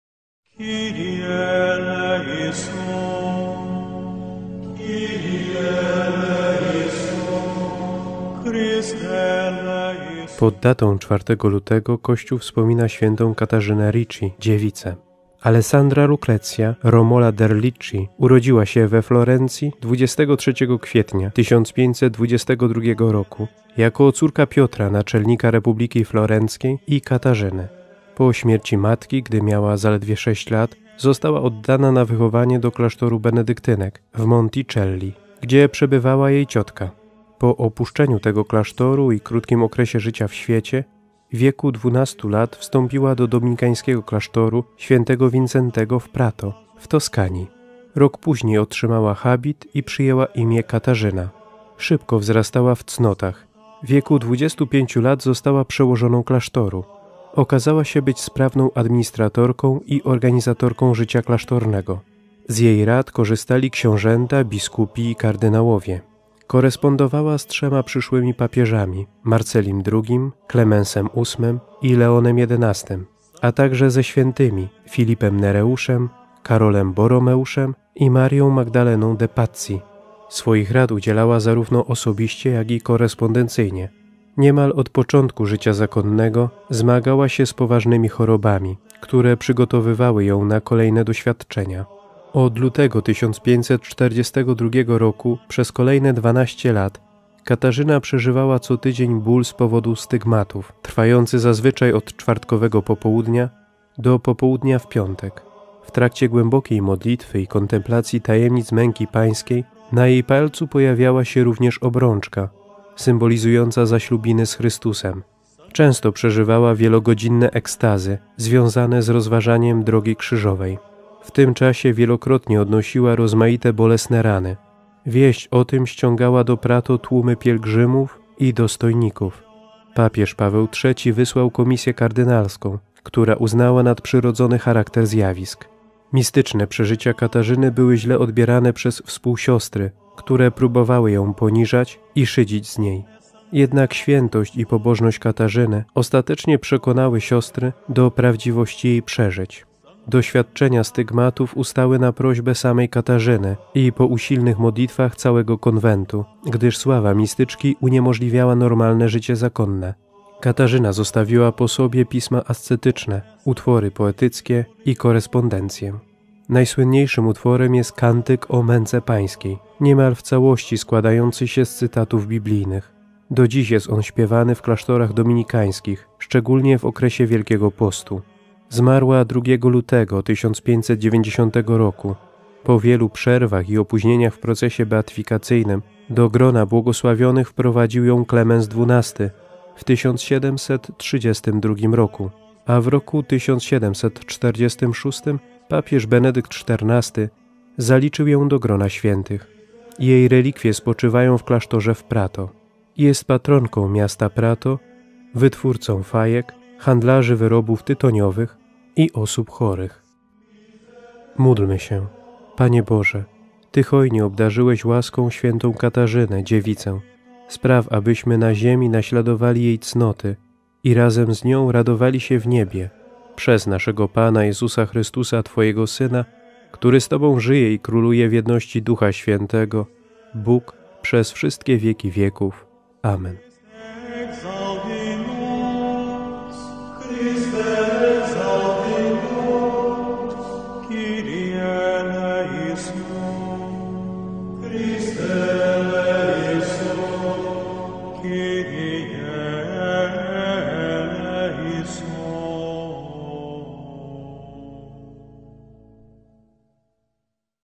Skarby Kościoła 4 luty | św. Katarzyna de' Ricci
Dnia 4 lutego wspominamy św. Katarzynę de' Ricci.